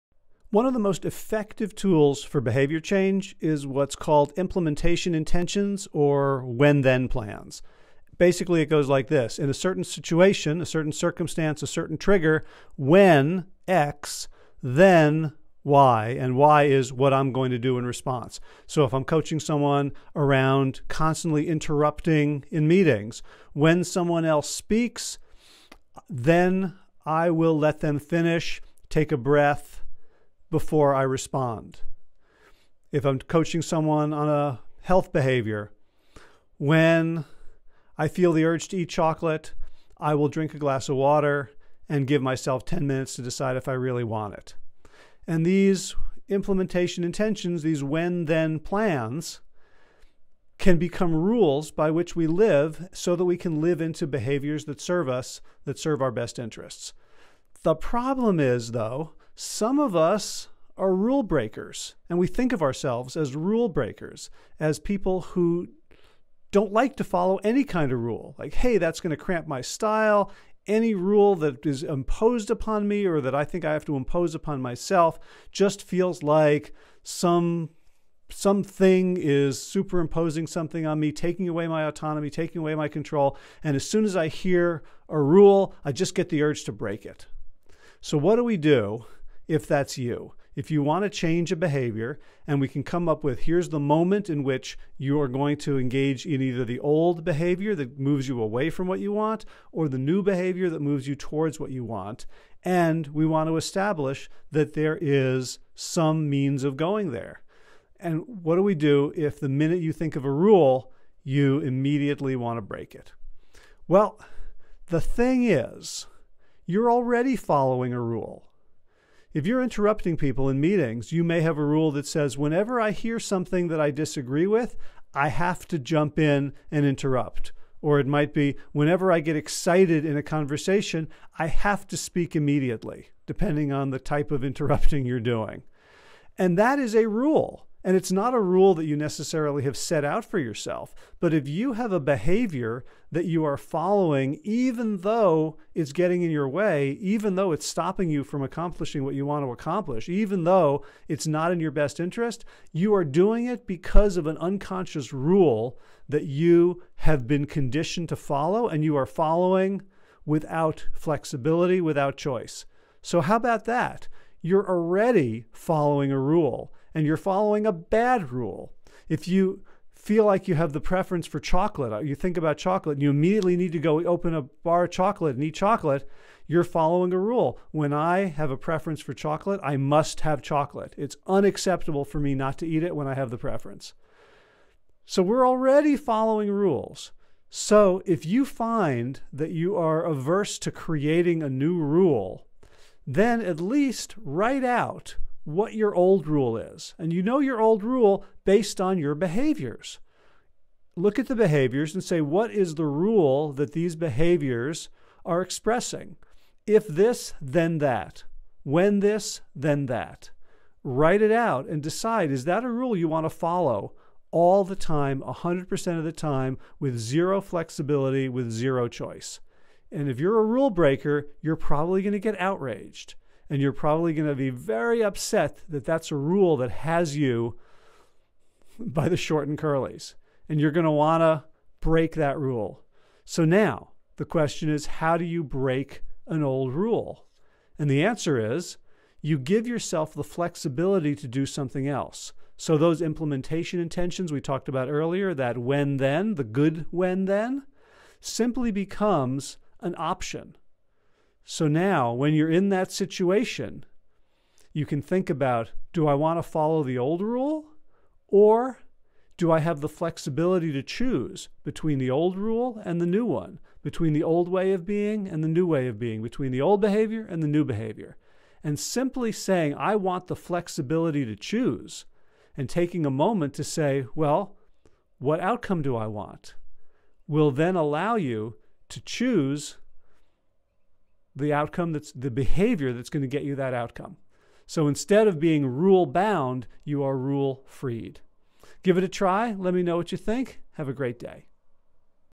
How to Change Your Dysfunctional Behaviors When You're a "Rule Breaker"
One of the most effective tools for behavior change is what's called "implementation intentions,"df
or when/then plans. If I'm coaching someone who's trying to break the habit of interrupting during meetings, they may develop a when/then plan that sounds like, "When someone else is speaking and I have the urge to interrupt, I will take a deep breath and let them finish."
These when/then plans can become rules that we live by, so we can act in ways that get us the outcomes we want in our lives.
The problem is that some of us are rule breakers, and we think of ourselves as people who don't like
to follow any kind of rule. We don't want to lose our autonomy or spontaneity. This is you if whenever you think of a rule, you just get a visceral desire to break it.
So what do you do if you want to change a behavior, but you aren't willing to create a rule about how to act?